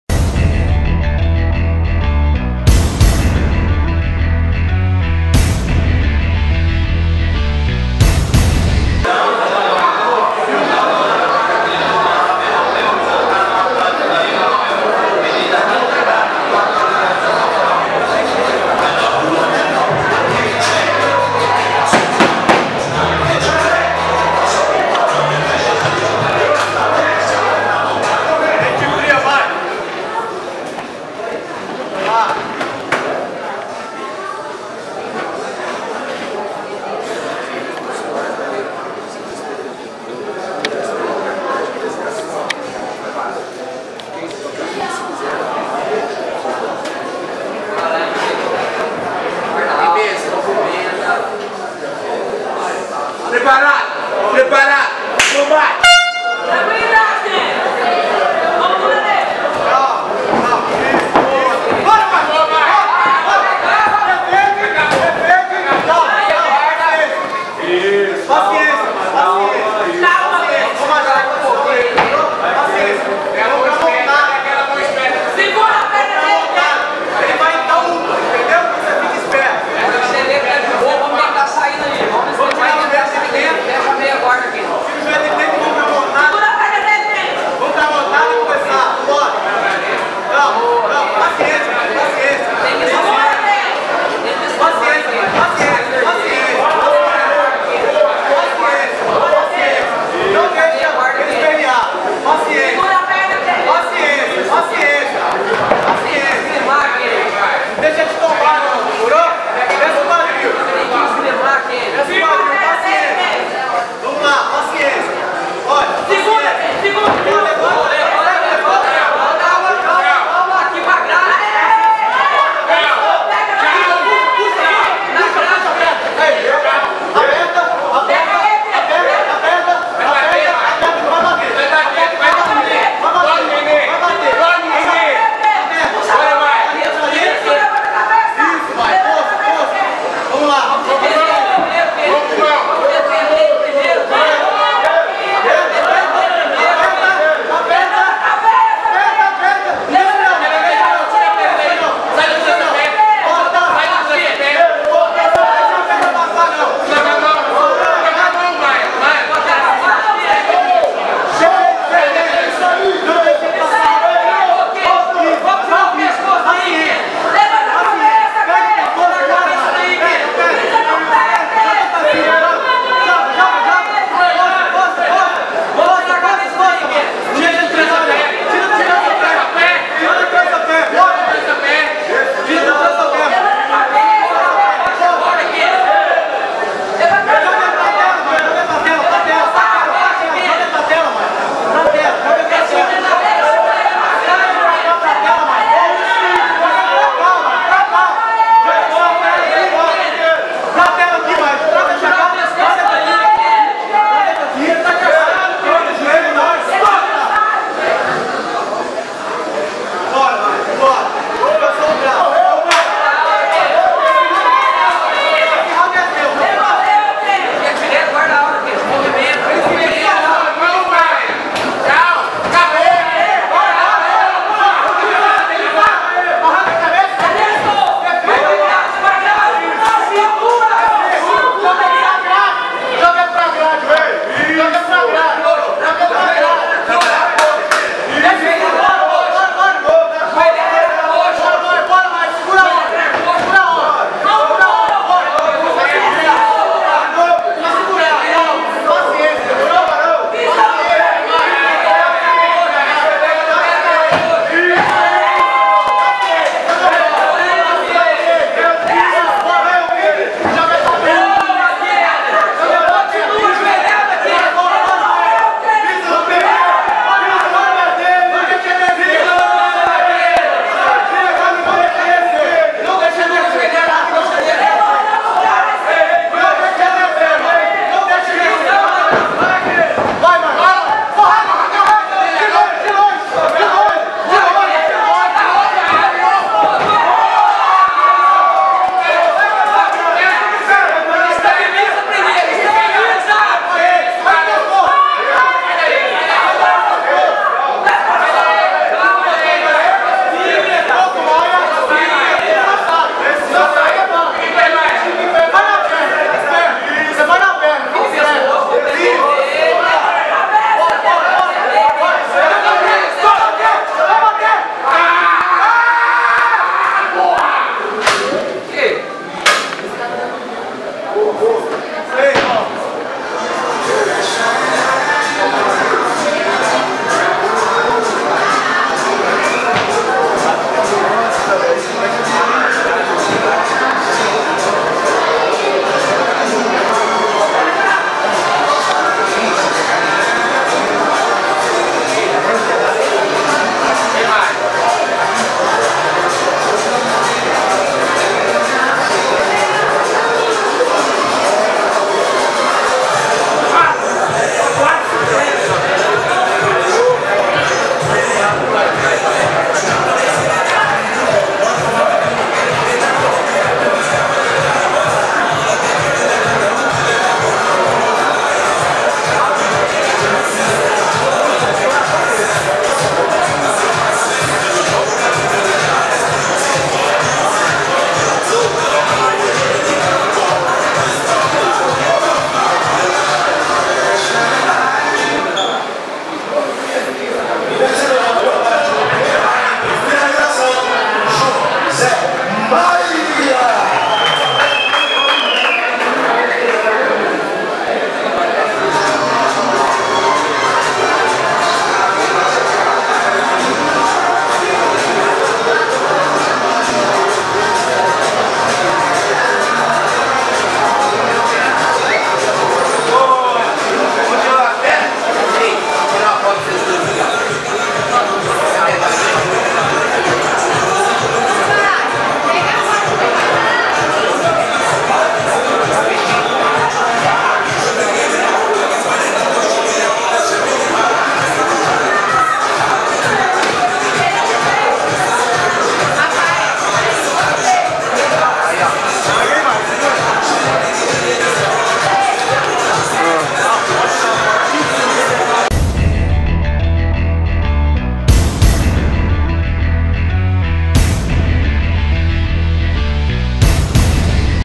José Maia vs Kennedy - FT Fight Championship 13
José Maia (Nathan Leite Fight Team) venceu Kennedy Mendes (Tchaco Fight Team) por finalização no primeiro round - 66kg.